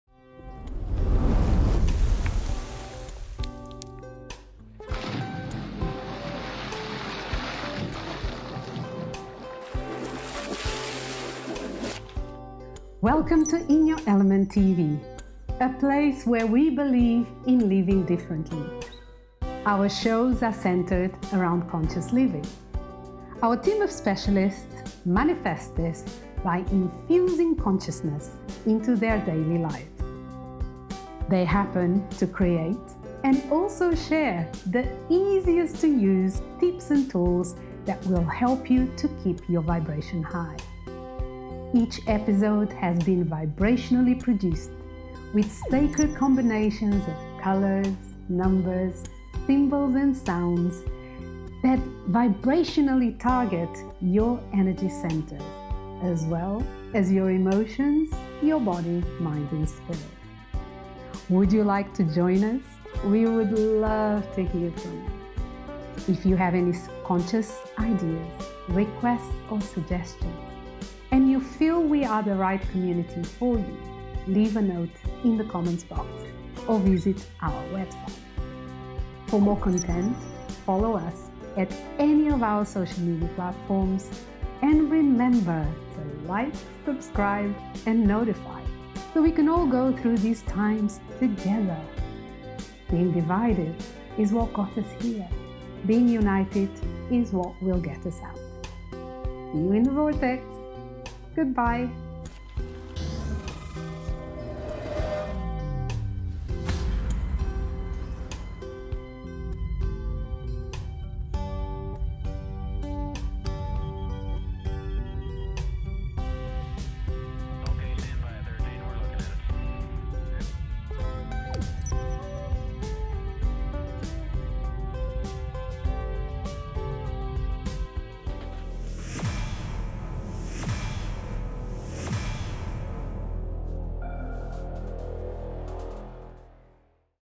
Welcome to IN YOUR ELEMENT TV
Welcome to In Your Element TV, a place where we believe in living differently.

Are you wondering WHAT ON EARTH IS GOING ON in a world where everything is happening and changing so fast?

❤️ Here at IN YOUR ELEMENT TV our shows are centred around CONSCIOUS LIVING to support you through these times of monumental changes.

💚 Our team of specialists share simple tips and tools to help you infuse consciousness into your day and keep your vibration high.

💜 Each episode has been vibrationally produced with SACRED combinations of colours, numbers, symbols and sounds that vibrationally target your energy centres as as well as your 4 elemental bodies.

“Being divided is what got us here, being united is what will get us out”

Join us so we can go through these times TOGETHER!

See you in the vortex!

Join our growing social communities, watch our videos, visit our IYE shop, admire our work and wish to financially support us:

2️⃣0️⃣2️⃣3️⃣ the year of Alchemy